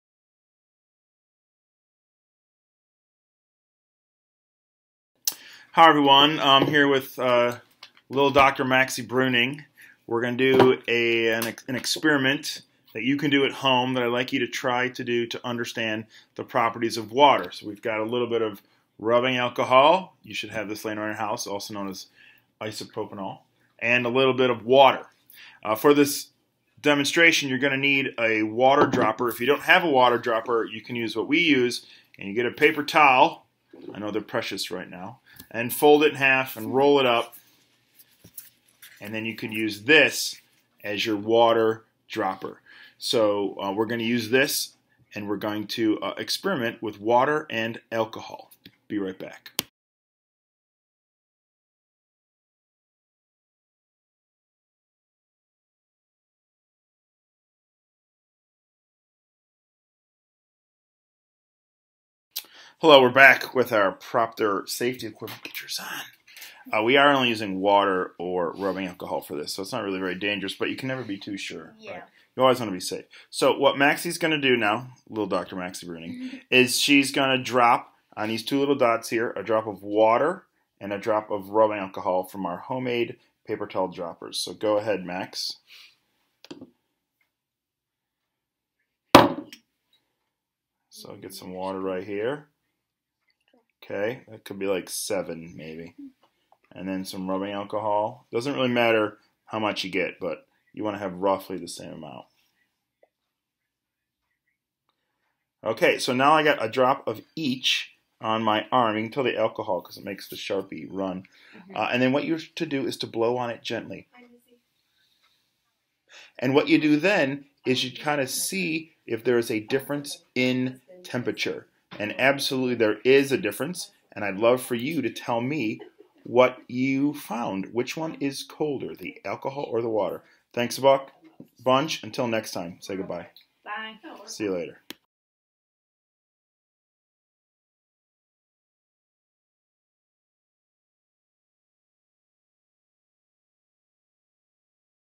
Evaporative cooling: Water vs Alcohol
This video asks students to compare a drop of water and a drop of alcohol (rubbing alcohol) on their arm to see which one feels colder. The purpose of this is to start a conversation about evaporative cooling within a greater discussion of the properties of water.